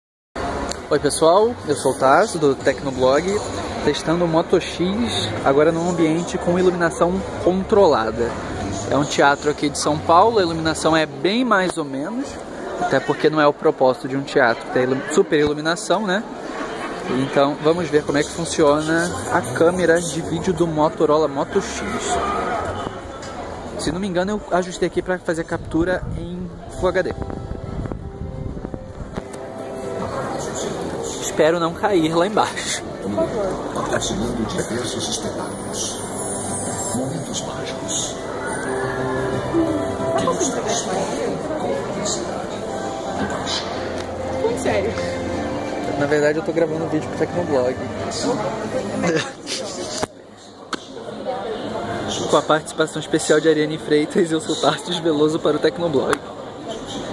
Motorola Moto X - Teste de câmera Full HD
Testamos a câmera de vídeo do Motorola Moto X num local com as piores condições que você poderia imaginar: um teatro. O local tem pouca iluminação, muito ruído e uma cortina supervermelha. Enfim, uma situação real na qual as pessoas usam o smartphone para registrar bons momentos.

Observações:
1. O flash LED não estava ligado.
2. O vídeo final em .mp4 tinha 126,2 MB.
3. Ele foi baixado do smartphone por meio do Android File Transfer para Mac e em seguida enviado para o YouTube sem qualquer edição.